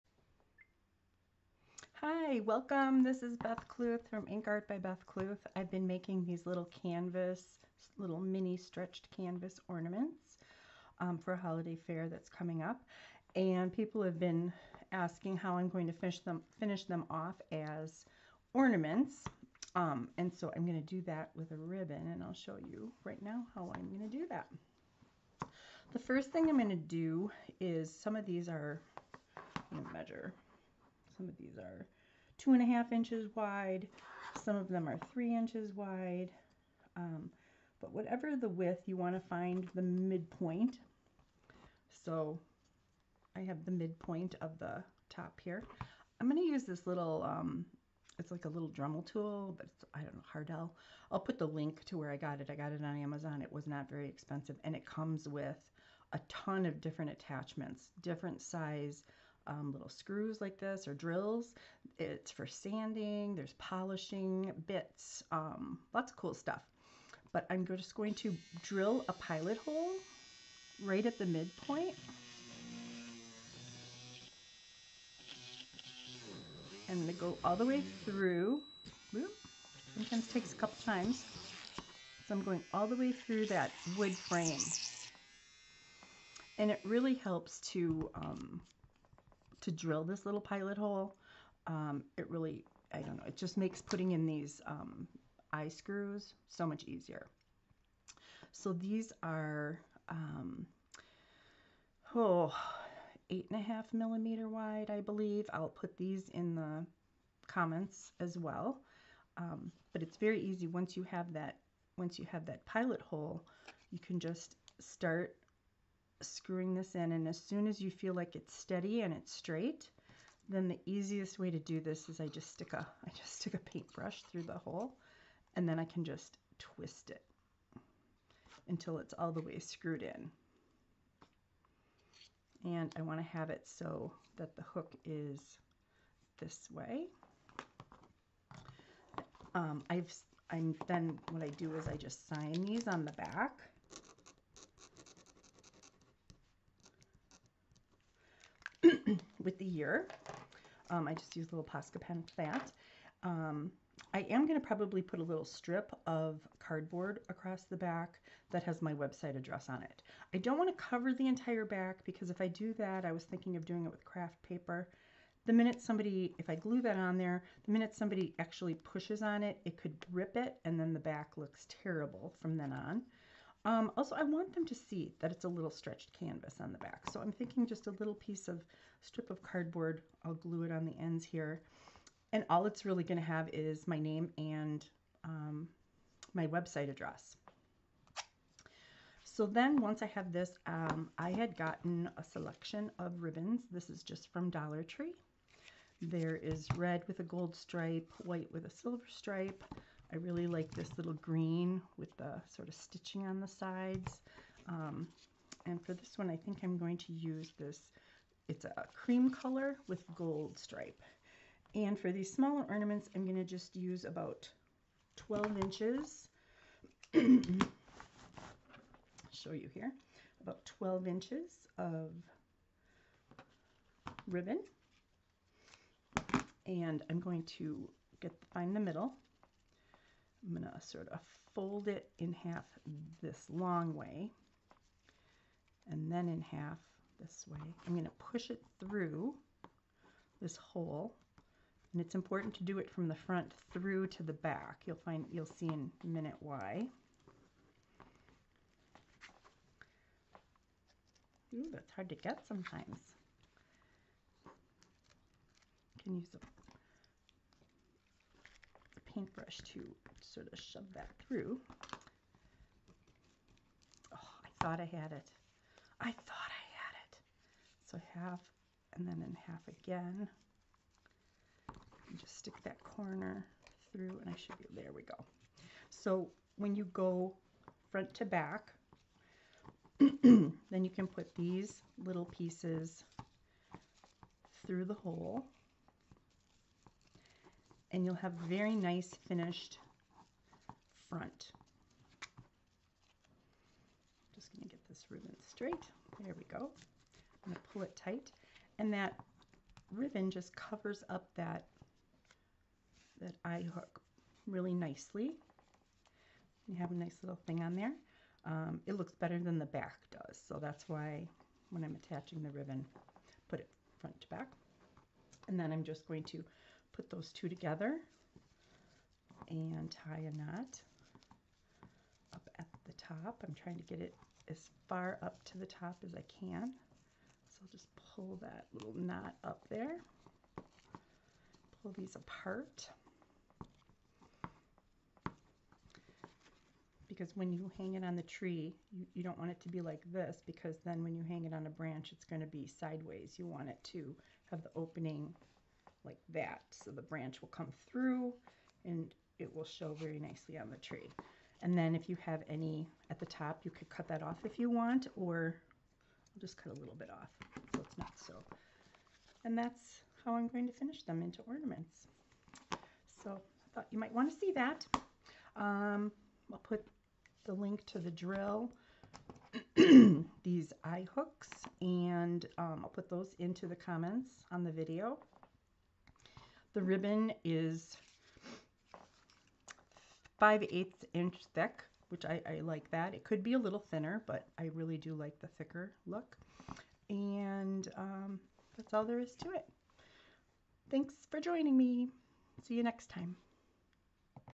How I turn mini canvases into ornaments
I'm making mini paintings on stretched canvases and turning them into ornaments for this holiday season. I posted several photos of the canvases and received many questions about how I plan to turn them into ornaments. This video demonstrates just how I do it.

Supplies used and mentioned in the video:
- Mini Stretched Canvas 3" X 4"
- Mini Stretched Canvas 3" x 3"
- Small Screw Eye Hooks
- Mini Cordless Rotary Tool